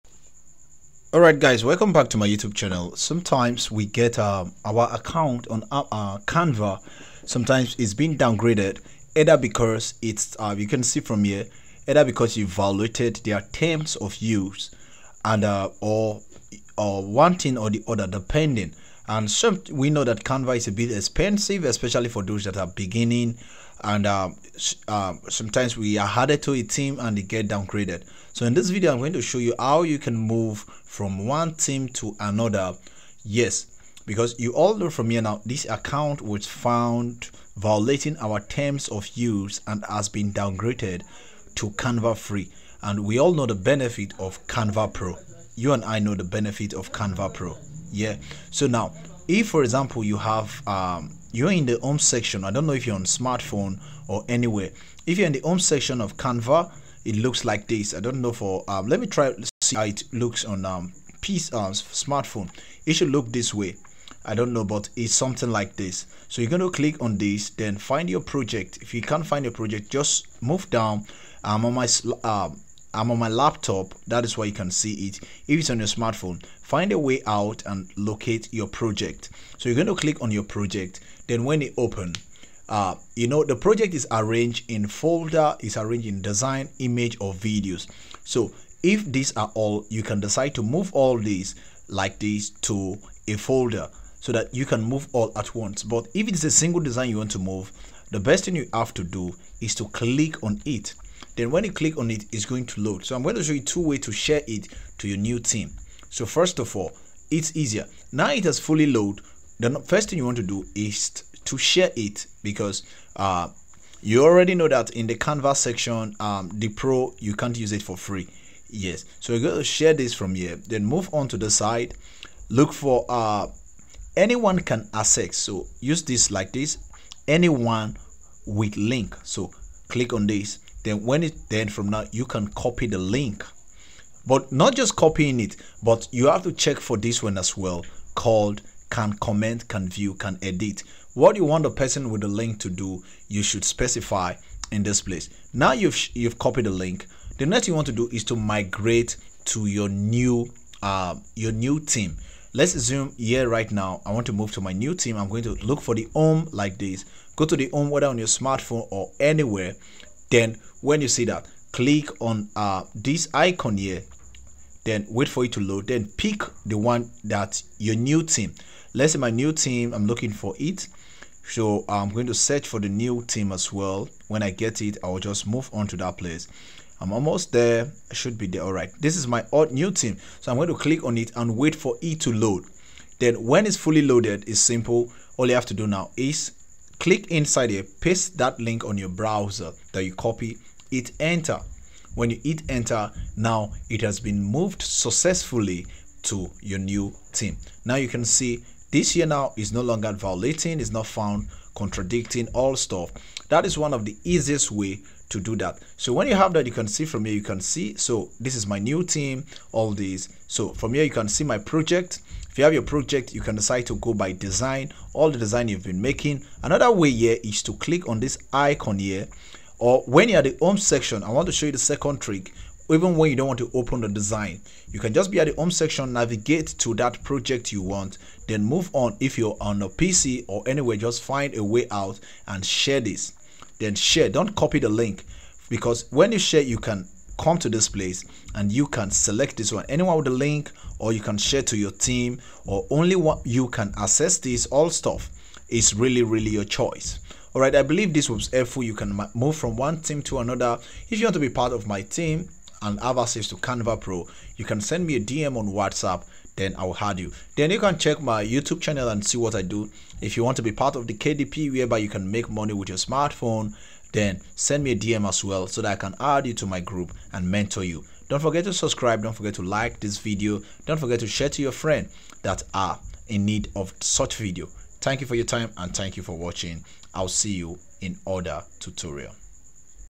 How to Transfer Canva Team//Design//Project From one Account to another || Share Canva Team Expired
sometimes, our Canva Team Get Expired or downgradedand we find it difficult to move from one Transfer our Project/Design from One Team to Another.

In this Video, I explain Two ways to Transfer your  canva Design/project/folder/templates from one canva Team to another.